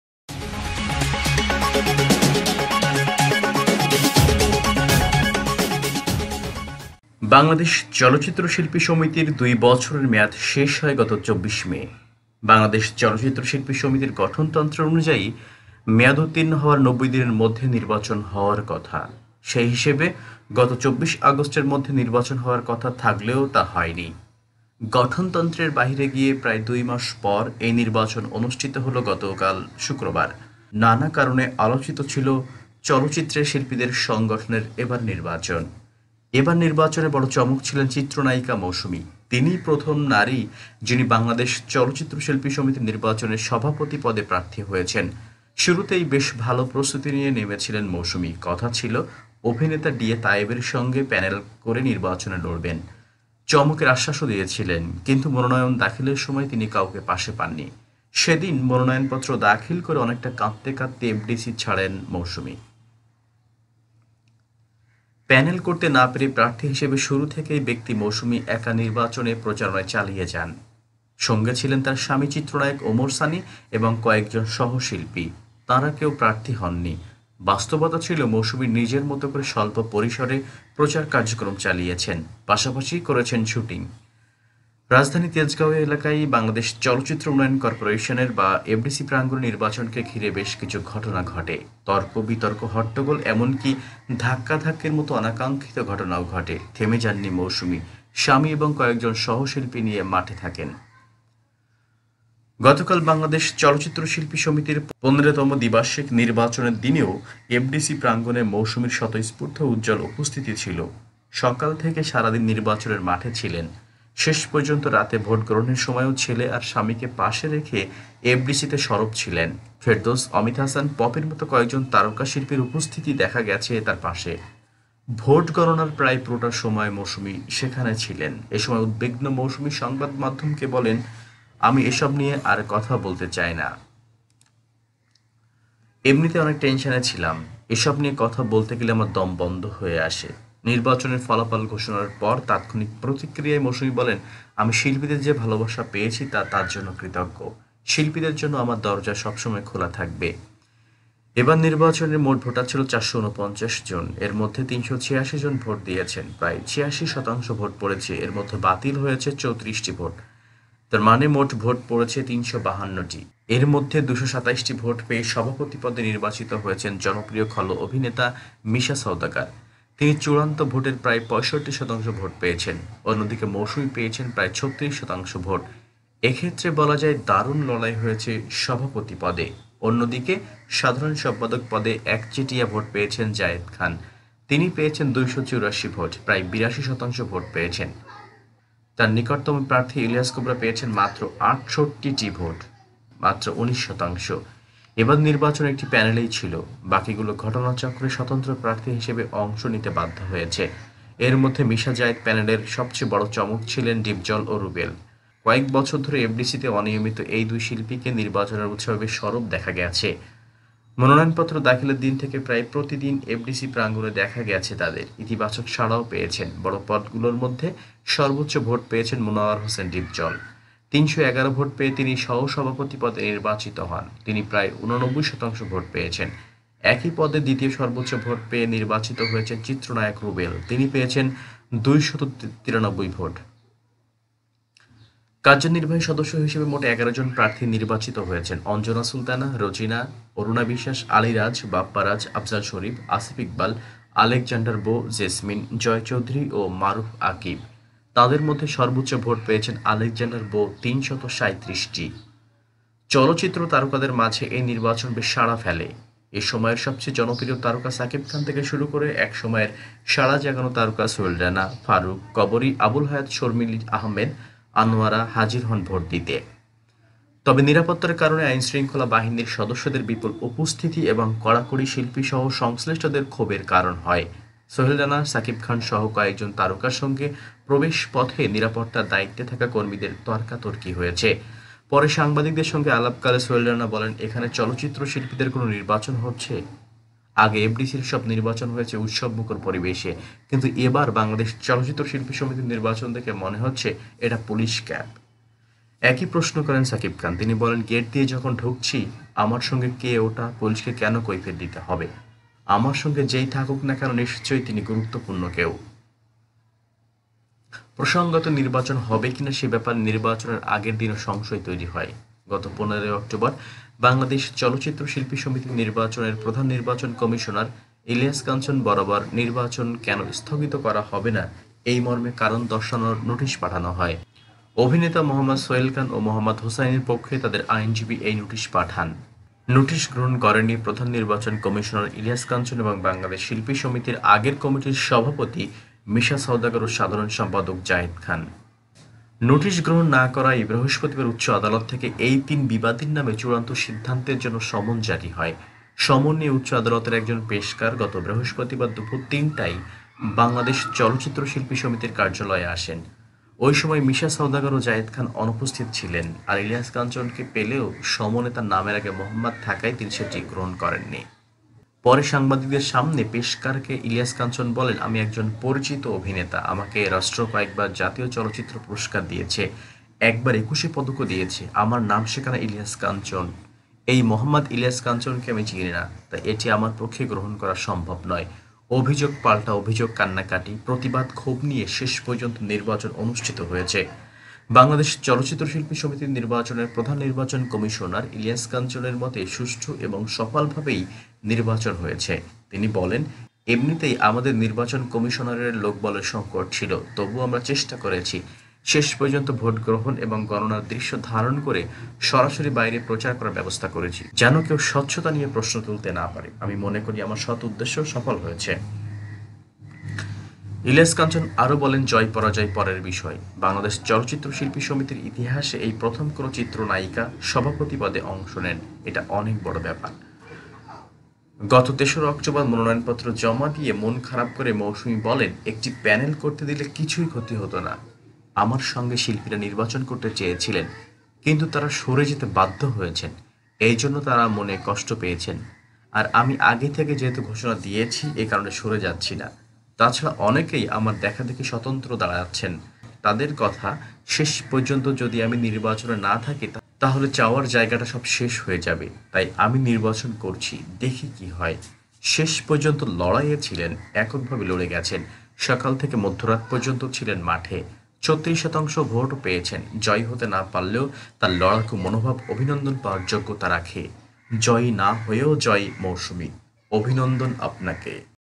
জয়ী না হয়েও ‘জয়ী’ মৌসুমী
বাংলাদেশ চলচ্চিত্র শিল্পী সমিতির দুই বছরের মেয়াদ শেষ হয় গত ২৪ মে। বাংলাদেশ চলচ্চিত্র শিল্পী সমিতির গঠনতন্ত্র অনুযায়ী মেয়াদোত্তীর্ণ হওয়ার ৯০ দিনের মধ্যে নির্বাচন হওয়ার কথা। সে হিসেবে গত ২৪ আগস্টের মধ্যে নির্বাচন হওয়ার কথা থাকলেও তা হয়নি। গঠনতন্ত্রের বাইরে গিয়ে প্রায় দুই মাস পর এই নির্বাচন অনুষ্ঠিত হলো গতকাল শুক্রবার। নানা কারণে আলোচিত ছিল চলচ্চিত্রের শিল্পীদের সংগঠনের এবারের নির্বাচন।
ভোট দিতে আসেন চিত্রনায়িকা পরীমনি। ছবি: প্রথম আলোএবার নির্বাচনে বড় চমক ছিলেন চিত্রনায়িকা মৌসুমী। তিনিই প্রথম নারী, যিনি বাংলাদেশ চলচ্চিত্র শিল্পী সমিতির নির্বাচনে সভাপতি পদে প্রার্থী হয়েছেন। শুরুতে বেশ ভালো প্রস্তুতি নিয়ে নেমেছিলেন মৌসুমী। কথা ছিল অভিনেতা ডি এ তায়েবের সঙ্গে প্যানেল করে নির্বাচনে লড়বেন। চমকের আশ্বাসও দিয়েছিলেন। কিন্তু মনোনয়ন দাখিলের সময় তিনি কাউকে পাশে পাননি। সেদিন মনোনয়ন পত্র দাখিল করে অনেকটা কাঁদতে কাঁদতেই এফডিসি ছাড়েন মৌসুমী।

প্যানেল করতে না পেরে প্রার্থী হিসেবে শুরু থেকেই ব্যক্তি মৌসুমী একা নির্বাচনের প্রচারণা চালিয়ে যান। সঙ্গে ছিলেন তাঁর স্বামী চিত্রনায়ক ওমর সানী এবং কয়েকজন সহশিল্পী। তাঁরা কেউ প্রার্থী হননি। বাস্তবতা ছিল, মৌসুমী নিজের মতো করে স্বল্প পরিসরে প্রচার কার্যক্রম চালিয়েছেন, পাশাপাশি শুটিং করেছেন।

রাজধানী তেজগাঁও এলাকার বাংলাদেশ চলচ্চিত্র উন্নয়ন করপোরেশনের বা এফডিসি প্রাঙ্গণে নির্বাচনকে ঘিরে বেশ কিছু ঘটনা ঘটে। তর্ক-বিতর্ক-হট্টগোল এমনকি ধাক্কাধাক্কির মতো অনাকাঙ্ক্ষিত ঘটনাও ঘটে। থেমে যাননি মৌসুমী। স্বামী এবং কয়েকজন সহশিল্পী নিয়ে মাঠে থাকেন।
নির্বাচনে দারুণ সাড়া ছিল শিল্পীদের মাঝে। ছবি: ফেসবুক থেকে নেওয়া।গতকাল বাংলাদেশ চলচ্চিত্র শিল্পী সমিতির ১৫তম দ্বিবার্ষিক নির্বাচনের দিনও এফডিসি প্রাঙ্গণে মৌসুমীর স্বতঃস্ফূর্ত উজ্জ্বল উপস্থিতি ছিল। সকাল থেকে সারা দিন নির্বাচনের মাঠে ছিলেন। শেষ পর্যন্ত রাতে ভোট গ্রহণের সময়ও ছেলে আর স্বামীকে পাশে রেখে এফডিসিতে সরব ছিলেন। ফেরদৌস, অমিত হাসান,পপির মতো কয়েকজন তারকা শিল্পীর উপস্থিতি দেখা গেছে তাঁর পাশে। ভোট গণনার প্রায় পুরোটা সময় মৌসুমী সেখানে ছিলেন। এ সময় উদ্বিগ্ন মৌসুমী সংবাদমাধ্যমকে বলেন, ‘আমি এসব নিয়ে আর কথা বলব না। এমনিতে অনেক টেনশনে ছিলাম। এসব নিয়ে কথা বলতে গেলে আমার দম বন্ধ হয়ে আসে।’

নির্বাচনের ফলাফল ঘোষণা পর তাৎক্ষণিক প্রতিক্রিয়ায় মৌসুমী বলেন, ‘আমি শিল্পীদের যে ভালোবাসা পেয়েছি, তার জন্য কৃতজ্ঞ। শিল্পীদের জন্য আমার দরজা সব সময় খোলা থাকবে।’
অরুণা বিশ্বাসের সেলফিতে শাবনাজ, নাঈম ও অমিত হাসান। ছবি: ফেসবুক থেকে নেওয়াএবার নির্বাচনে মোট ভোটার ছিল ৪৪৯ জন। এর মধ্যে ৩৮৬ জন ভোট দিয়েছেন। প্রায় ৮৬ শতাংশ ভোট পড়েছে। এর মধ্যে বাতিল হয়েছে ৩৪টি ভোট। তার মানে মোট ৩৫২টি ভোটে ফলাফল নির্ধারিত হয়েছে।
সভাপতি পদে প্রতিদ্বন্দিতা করেছেন চিত্রনায়িকা মৌসুমী। ছবি: প্রথম আলোএর মধ্যে ২২৭টি ভোট পেয়ে সভাপতি পদে নির্বাচিত হয়েছেন জনপ্রিয় খল অভিনেতা মিশা সওদাগর। তিনি চূড়ান্ত ভোটের প্রায় ৬৫ শতাংশ ভোট পেয়েছেন। অন্যদিকে মৌসুমী পেয়েছেন প্রায় ৩৬ শতাংশ ভোট। এ ক্ষেত্রে বলা যায়, দারুণ লড়াই হয়েছে সভাপতি পদে। অন্যদিকে সাধারণ সম্পাদক পদে একচেটিয়া ভোট পেয়েছেন জায়েদ খান। তিনি পেয়েছেন ২৮৪টি ভোট। প্রায় ৮২ শতাংশ ভোট পেয়েছেন। তাঁর নিকটতম প্রার্থী ইলিয়াস কোবরা পেয়েছেন মাত্র ৬৮টি ভোট, মাত্র ১৯ শতাংশ।
এসেছিলেন চিত্রনায়িকা চম্পা। ছবি: প্রথম আলোএবার নির্বাচনে একটি প্যানেলই ছিল। বাকিগুলো ঘটনাচক্রে স্বতন্ত্র প্রার্থী হিসেবে অংশ নিতে বাধ্য হয়েছে। এর মধ্যে মিশা-জায়েদ প্যানেলের সবচেয়ে বড় চমক ছিলেন ডিপজল ও রুবেল। কয়েক বছর ধরে এফডিসিতে অনিয়মিত এই দুই শিল্পীকে নির্বাচনের উৎসবে বেশ সরব দেখা গেছে। মনোনয়নপত্র দাখিলের দিন থেকে প্রায় প্রতিদিন এফডিসি প্রাঙ্গণে দেখা গেছে তাঁদের। ইতিবাচক সাড়াও পেয়েছেন। বড় পদগুলোর মধ্যে সর্বোচ্চ ভোট পেয়েছেন মনোয়ার হোসেন ডিপজল। ৩১১ ভোট পেয়ে তিনি সহসভাপতি পদে নির্বাচিত হন। তিনি প্রায় ৮৯ শতাংশ ভোট পেয়েছেন। একই পদে দ্বিতীয় সর্বোচ্চ ভোট পেয়ে নির্বাচিত হয়েছেন চিত্রনায়ক রুবেল। তিনি পেয়েছেন ২৯৩ ভোট।

কার্যনির্বাহী সদস্য হিসেবে মোট ১১ জন প্রার্থী নির্বাচিত হয়েছেন। অঞ্জনা সুলতানা, রোজিনা, অরুণা বিশ্বাস, আলীরাজ, বাপ্পারাজ, আফজাল শরীফ, আসিফ ইকবাল, আলেকজান্ডার বো, জেসমিন, জয় চৌধুরী ও মারুফ আকিব। তাঁদের মধ্যে সর্বোচ্চ ভোট পেয়েছেন আলেকজান্ডার বো, ৩৩৭টি।